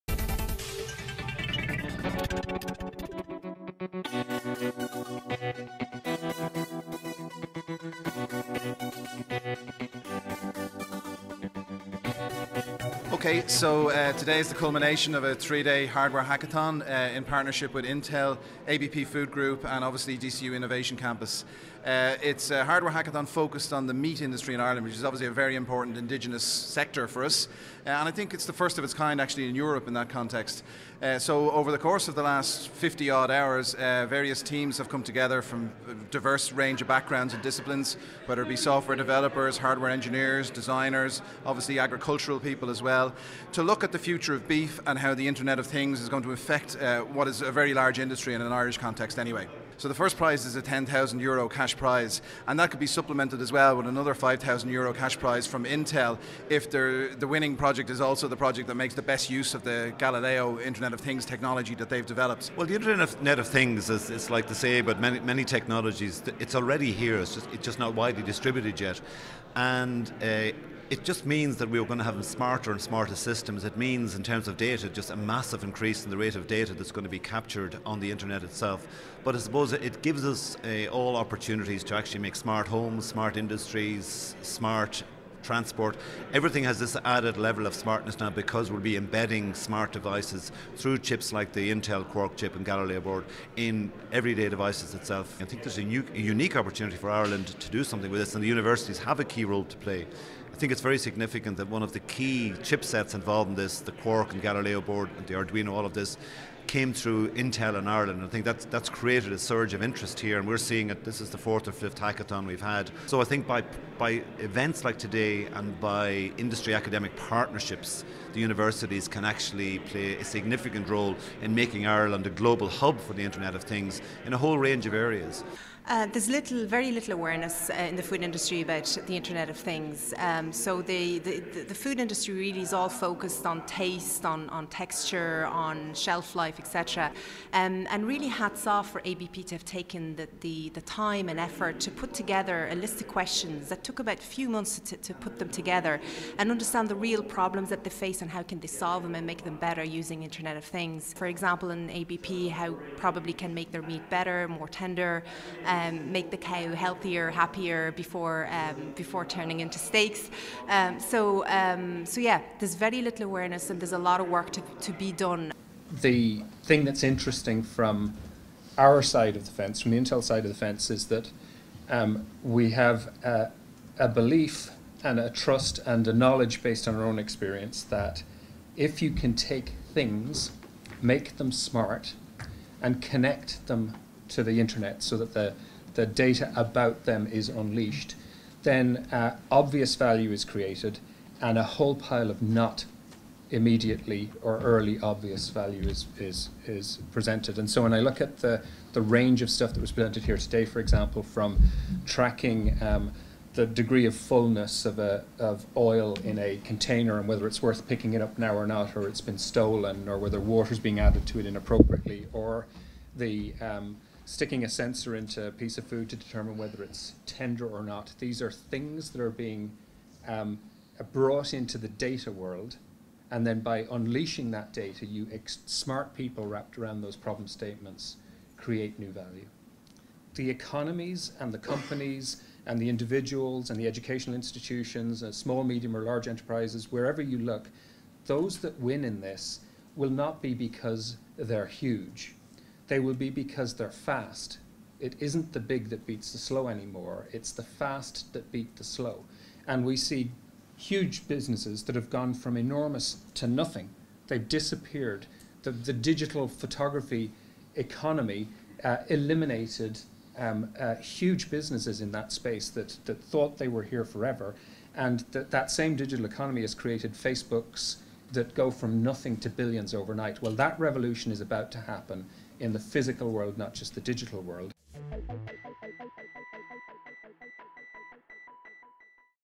Intel and DCU host first beef hackathon
Ireland’s first beef hackathon, which aimed to bring the internet of things (IoT) to the Irish beef industry under a partnership with ABP Food Group, Intel and Dublin City University (DCU), took place 20-22 March on DCU’s Innovation Campus.
Graduates of all disciplines with particular emphasis on agriculture, food technology, innovation and design, engineering and software development took part in the event.
As one of Ireland’s traditional industries, agriculture, and beef in particular, is gradually embracing technological change and IoT is one that is being hotly tipped to not just increase efficiency among beef production, but this in turn will increase annual yields at a time when beef demands worldwide are increasing at an exponential rate.